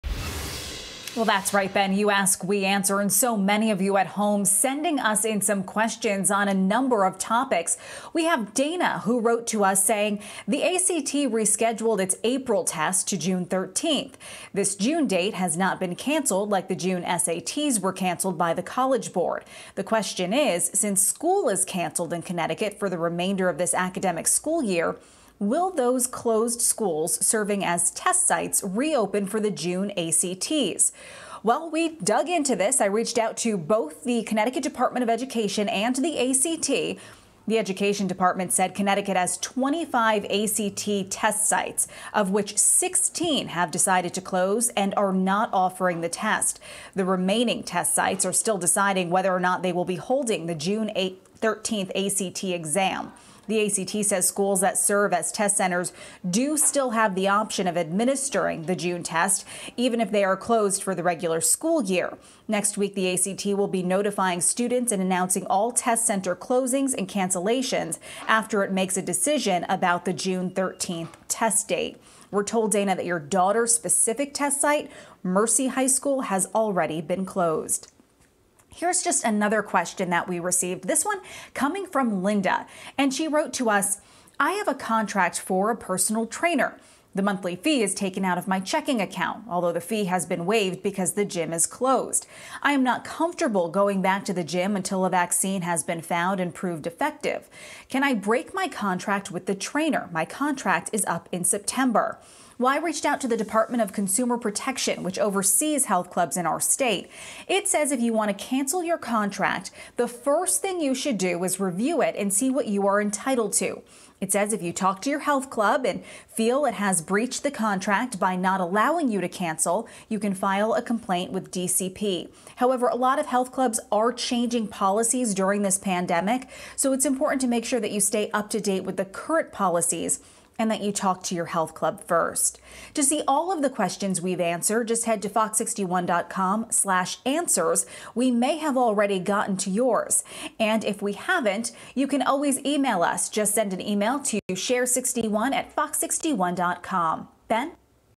You Ask. We Answer. | ACT testing postponed; Cancelling a personal trainer contract
"I am not comfortable going back to the gym until a vaccine has been found and proved effective. Can I break my contract with the trainer?"